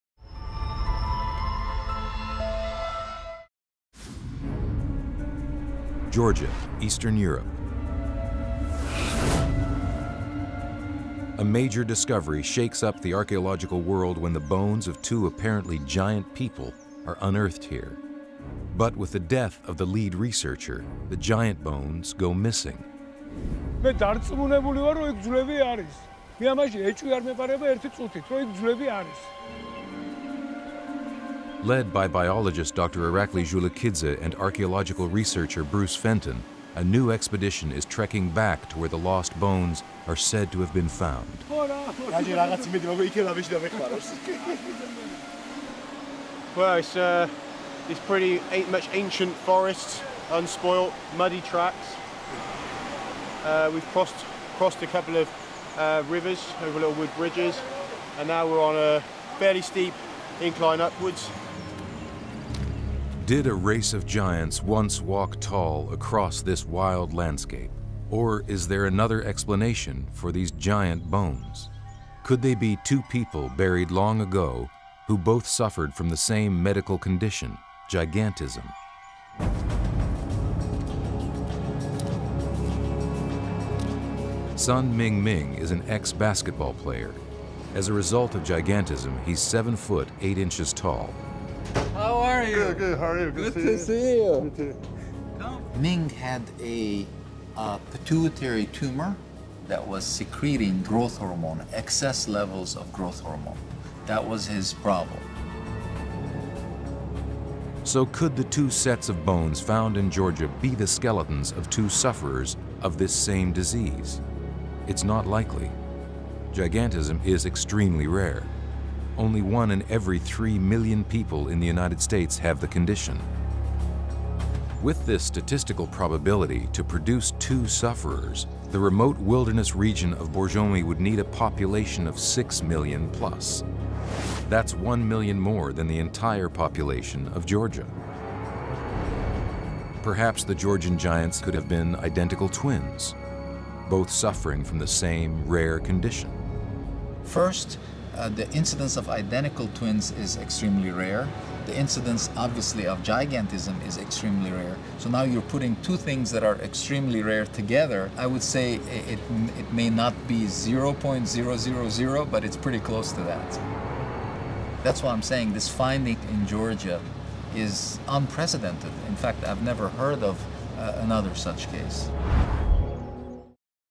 The Lost Giants of Georgia
A discovery of bones belonging to seemingly giant people raise questions. Research looks to the real giants that live among us.